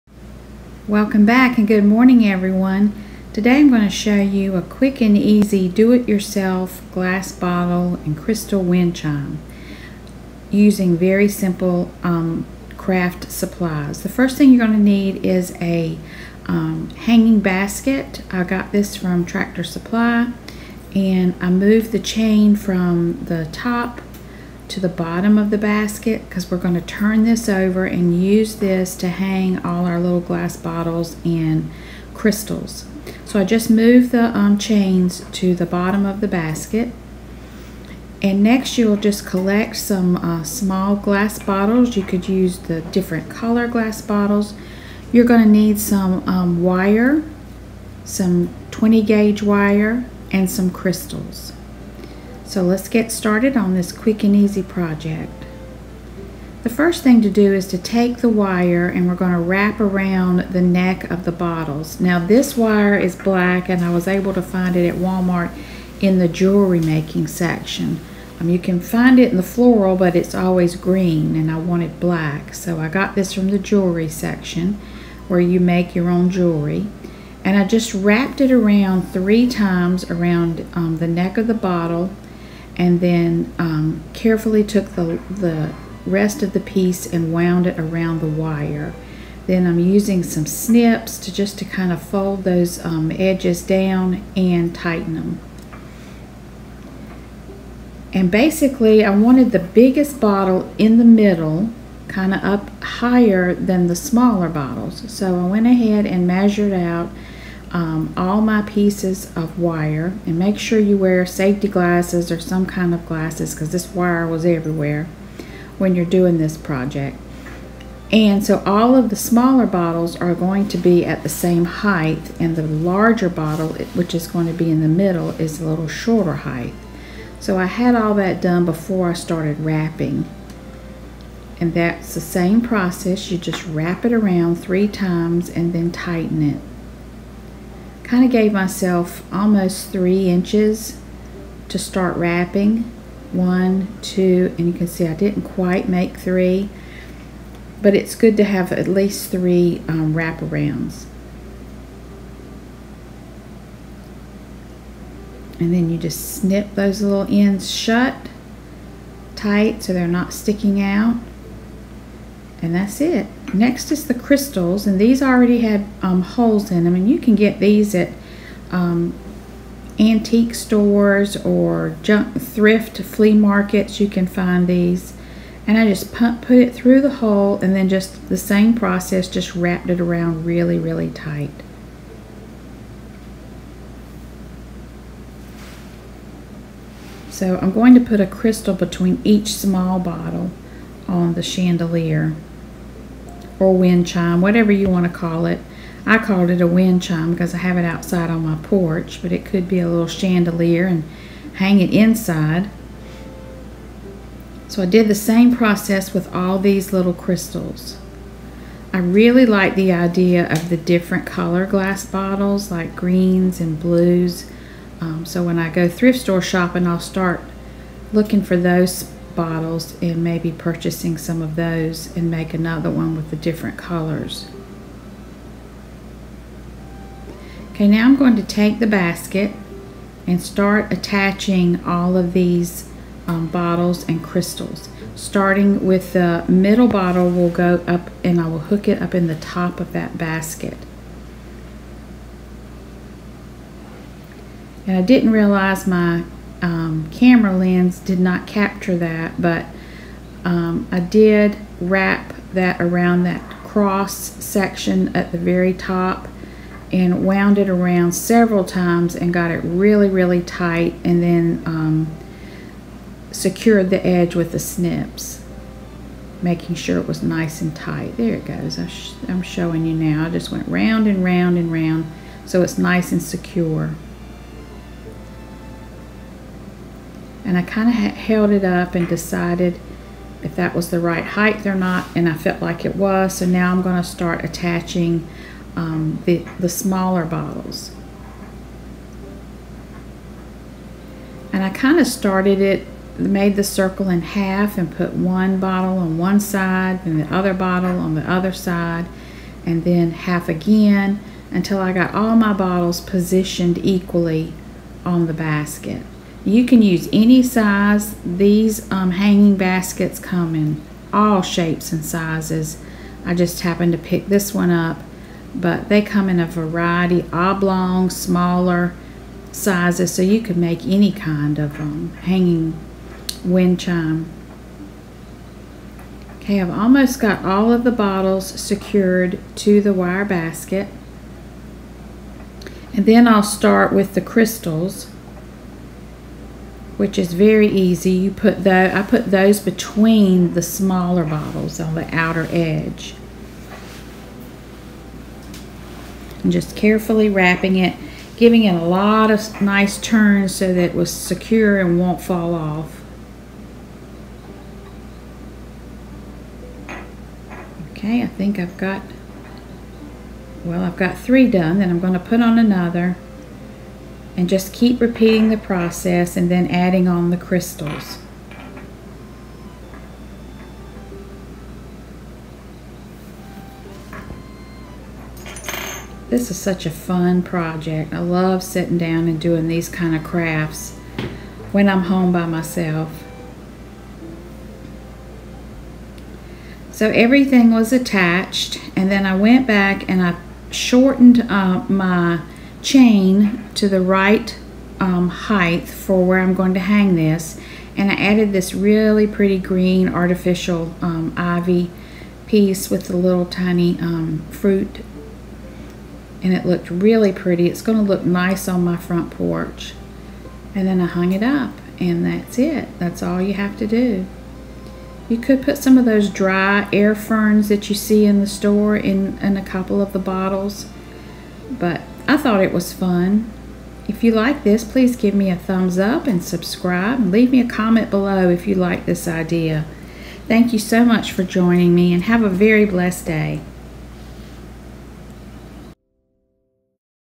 DIY Glass Bottle Wind Chime
Step by step instructions on making a glass bottle and crystal wind chime using a wire hanging basket.  Fun and easy craft project.